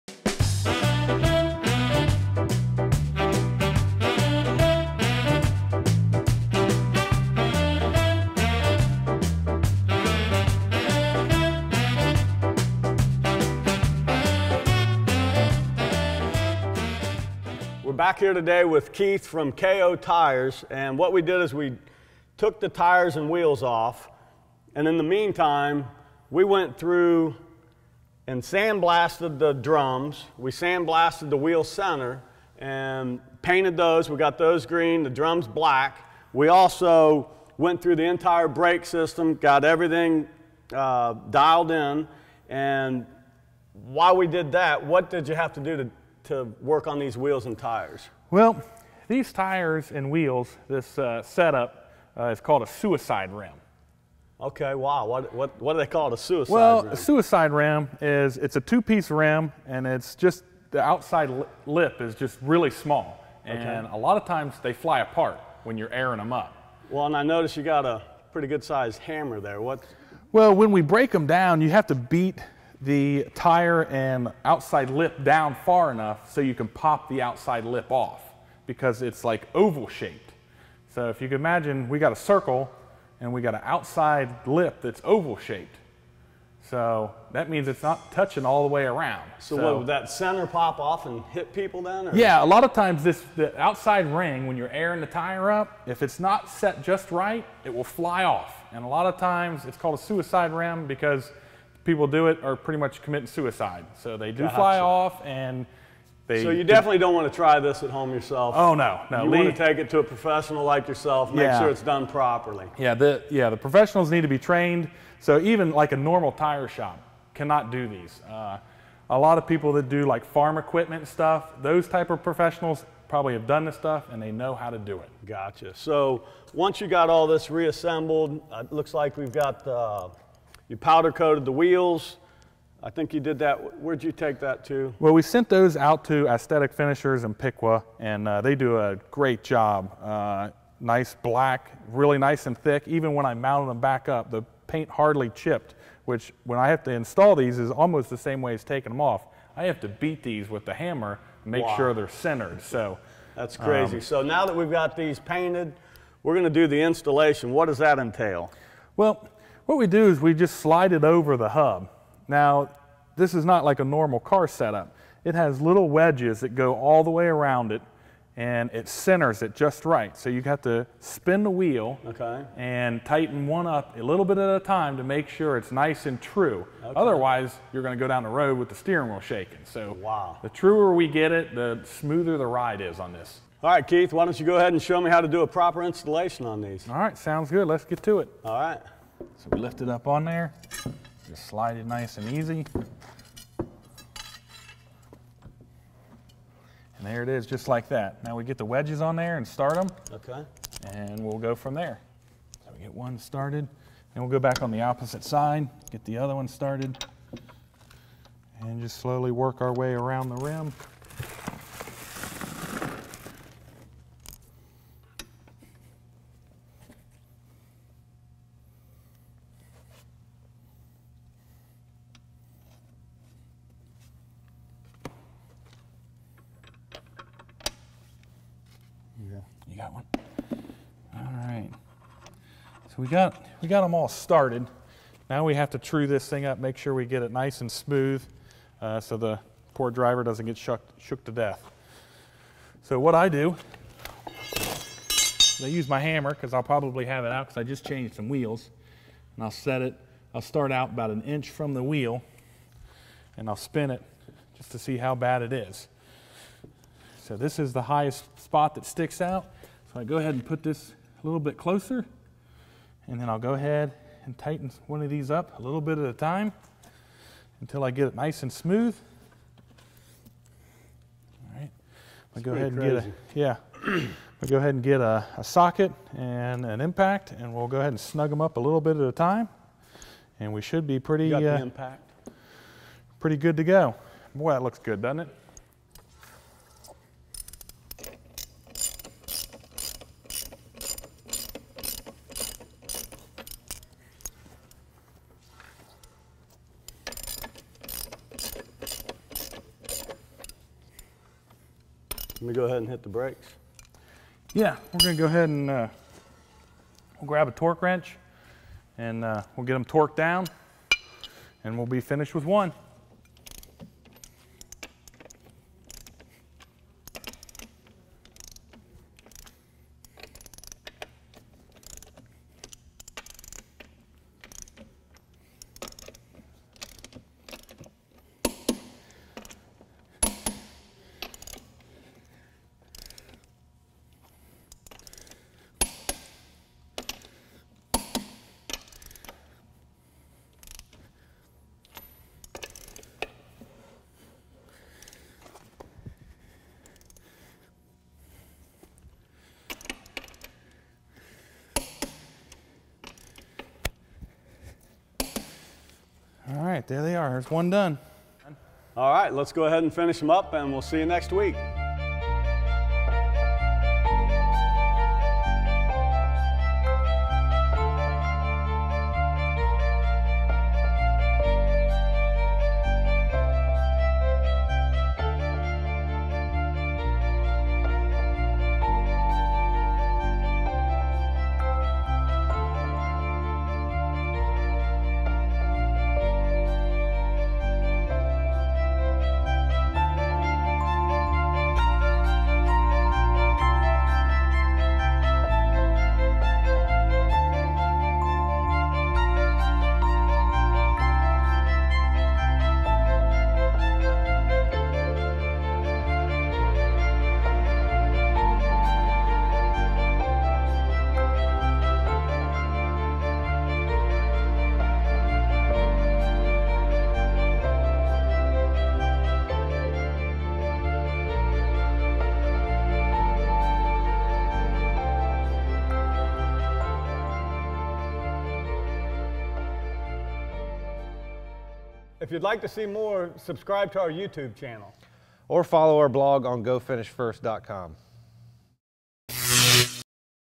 Split Rims Deadly Suicide Rims; Split Ring
The Diamond T Split Rims were originally designed for combat trucks so that flat tires were easily changed in the field. But due to poor design they became very unstable over time and were quickly labeled “Widow Makers” or Suicide Rims” as a result of the outer rim exploding from the tire during inflation, often resulting in serious injury and even death.

We sent our Split Rings out to a tire service center located in the country since they have experience dealing with farm vehicles that still use these type of Split Ring Rims. Lets watch Mark as he and Keith with KO Tire talk about dangers of these types of rims and watch as they install the repaired tires for the Diamond T.

For more information about Finish 1st Abrasive products, please visit our